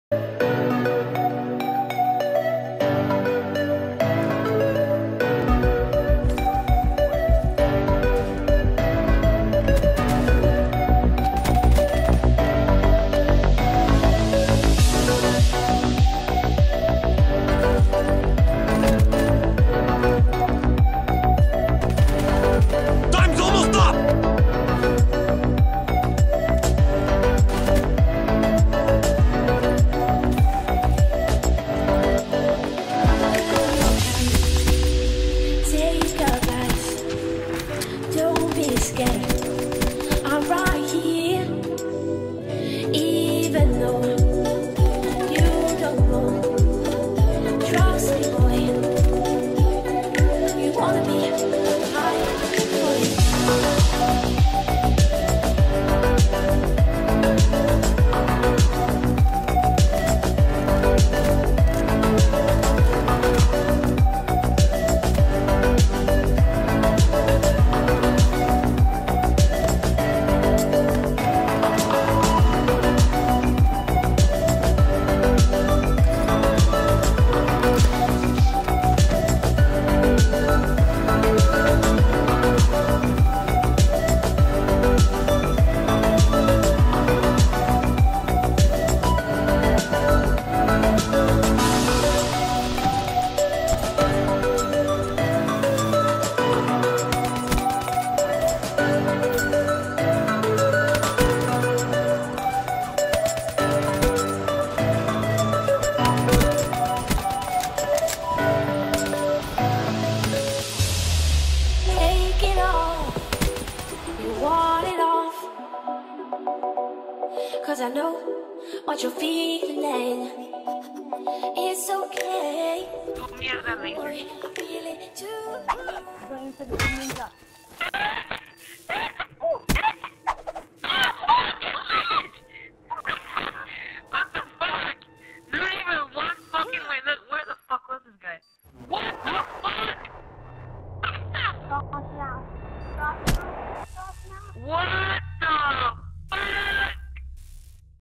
Monthly Montage # 1 ( Sick S&D Clutches, Sick Kill cams,Feeds and More!! )
What Is going on guys and in today's video is just my monthly montage. I grabbed all the best  clips I got and put them Into this video! Hope you enjoy:)